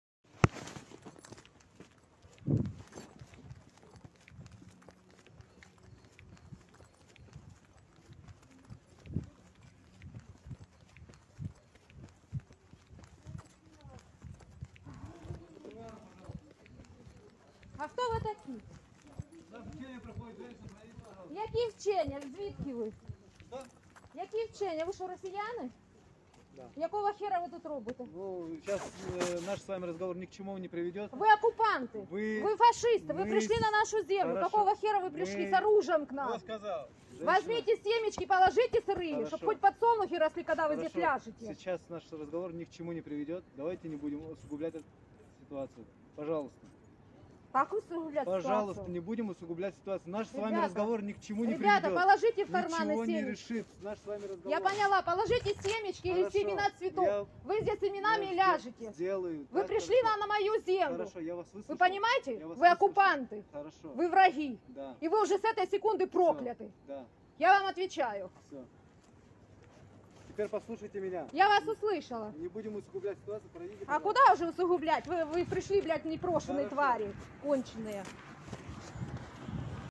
War in Ukraine- если у нас такие смелые женщины то что уже говорить за нас Мужчин
Российские оккупационные войска вошли в город Геническ.

если у нас такие смелые женщины то что уже говорить за нас Мужчин- тому пример наши герои на острове Змеиный- вечная память